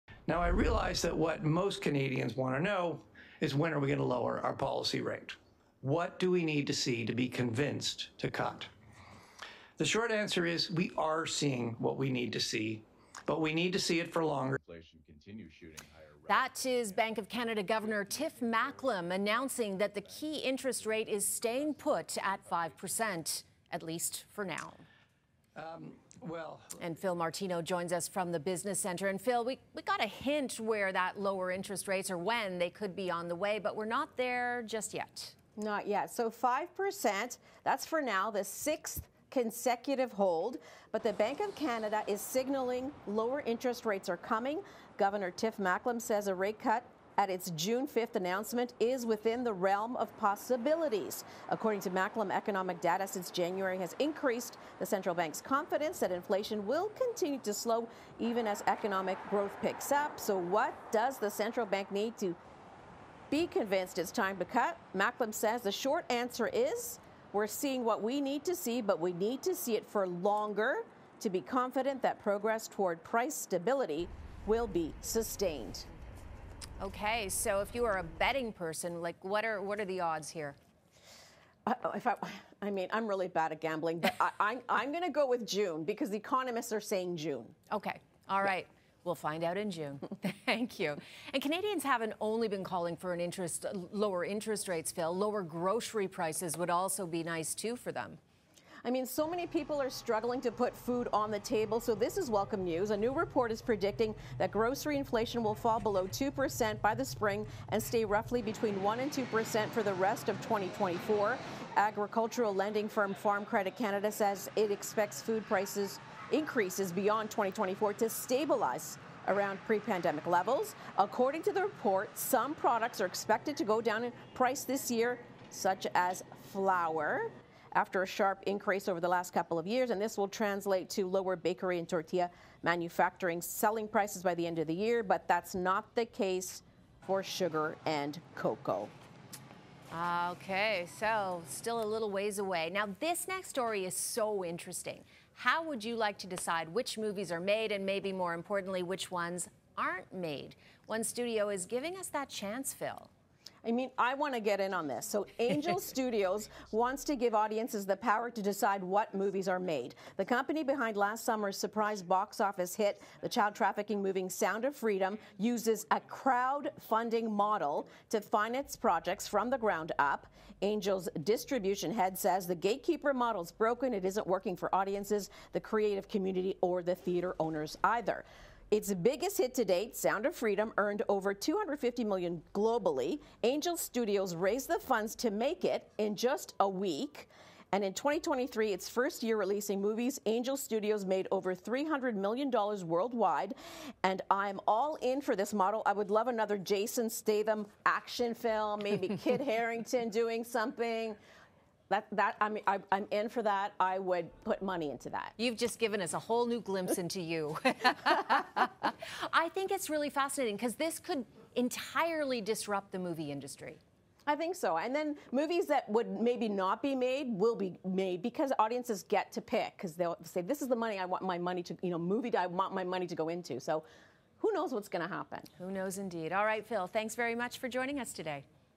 Business Report: Interest rate cuts starting soon?
Canada's key interest rate remains unchanged, but we're getting hints at when cuts could begin. Plus, grocery prices may be stabilizing and movie-goers could get a say in which films are made. Fil Martino explains.